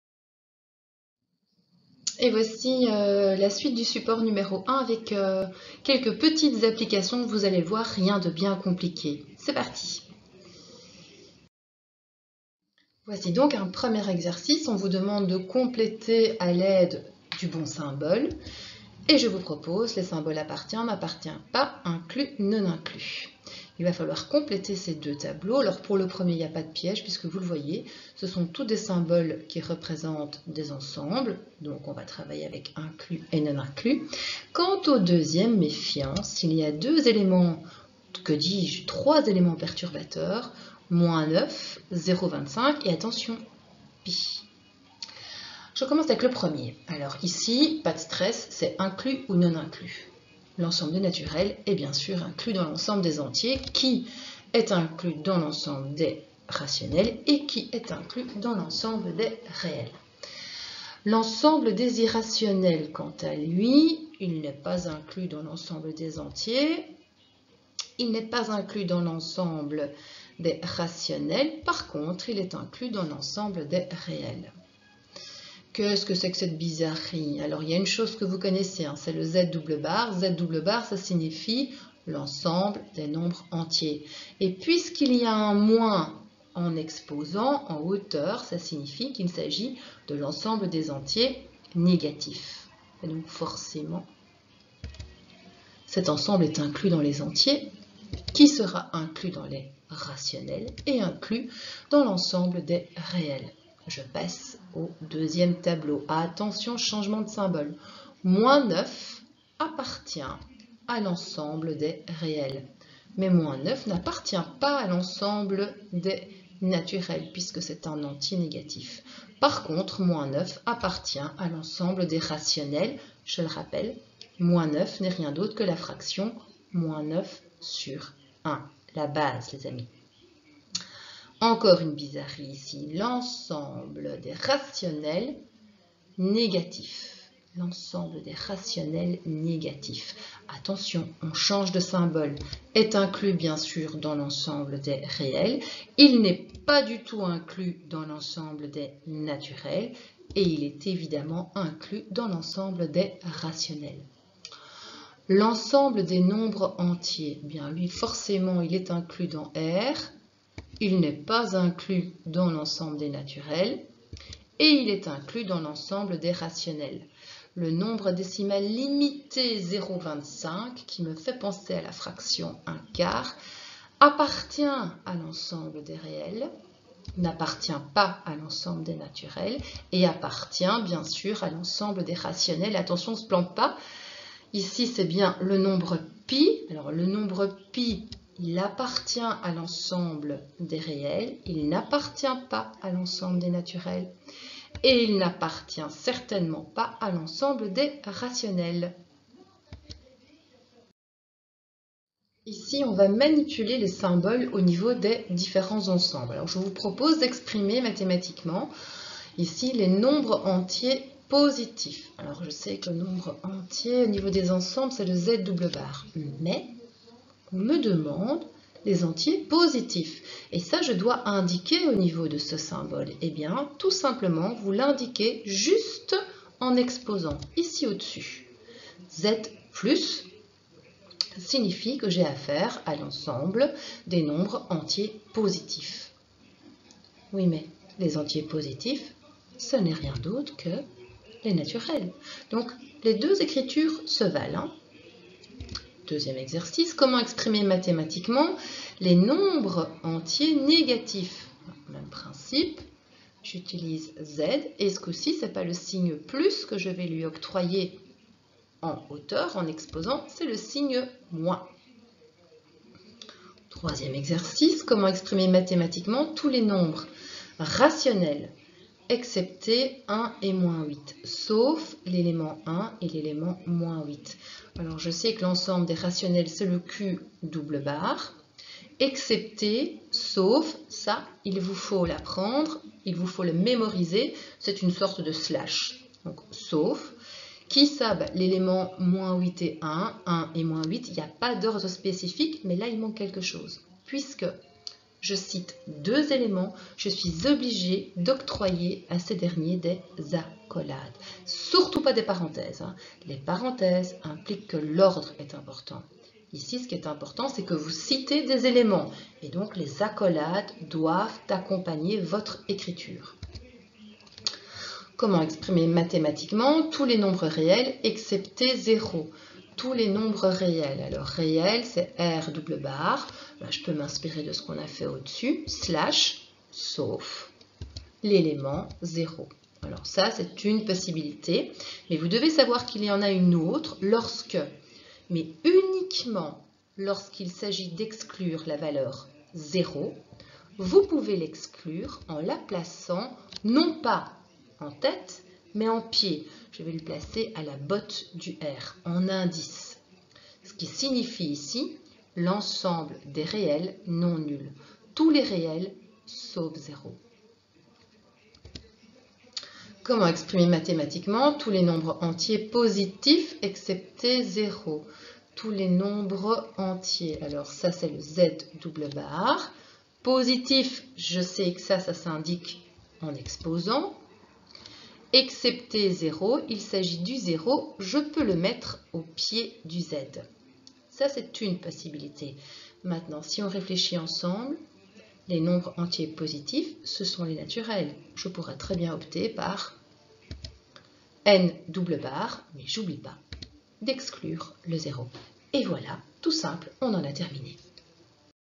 (1.1) Les Différents Ensembles de Nombres -Applications
Différence entre les symboles liés aux ensembles :
  "appartient & n'appartient pas"
 "est inclus dans.. ou n'est pas inclus dans.."